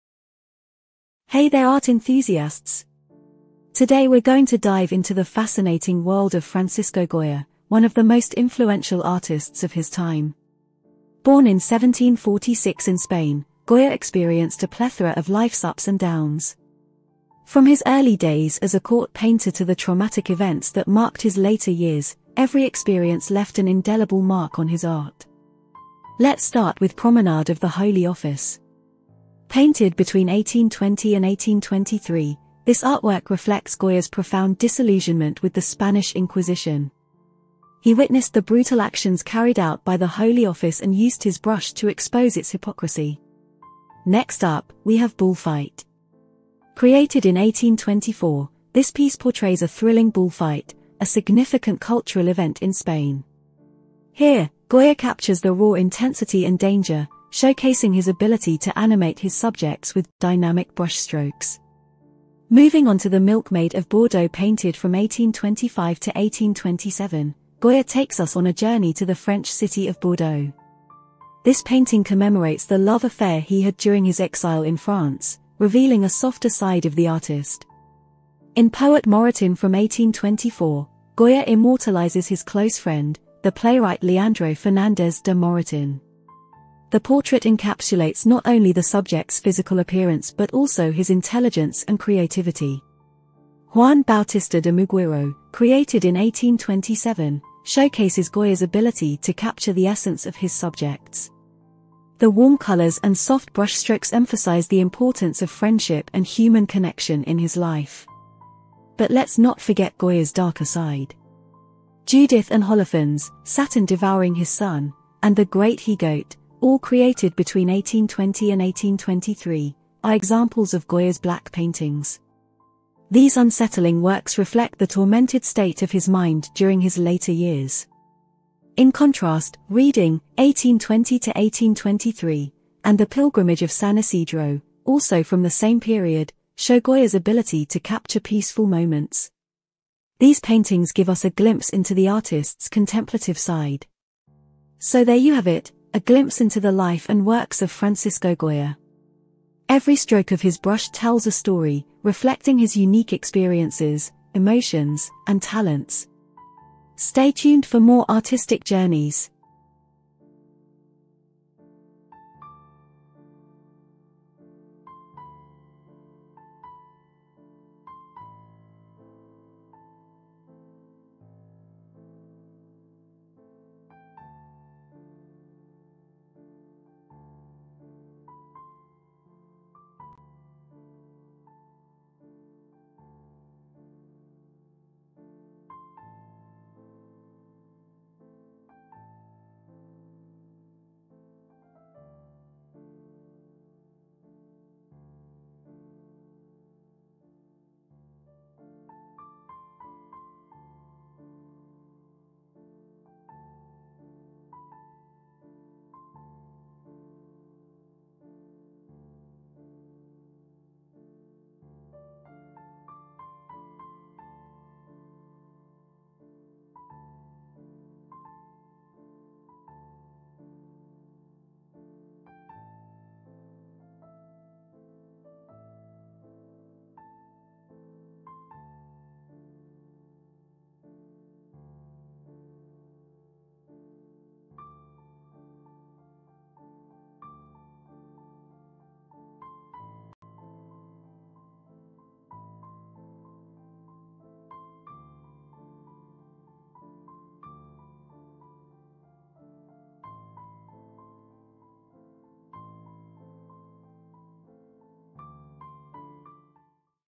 Francisco Goya: A collection of 10 oil paintings with title and year, 1820-1827 [HD]
Francisco Goya: oil paintings, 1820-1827
Promenade Of The Holy Office, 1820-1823
Bullfight, 1824
The Milkmaid Of Bordeaux, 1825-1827
Poet Moratín, 1824
Juan Bautista De Muguiro, 1827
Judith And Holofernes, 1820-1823
Saturn Devouring His Son, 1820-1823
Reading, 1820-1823
The Pilgrimage Of San Isidro, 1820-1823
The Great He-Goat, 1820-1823
OnlineArtGallery,CollectionOfPaintings,ArtHistory,FranciscoGoya
9c7f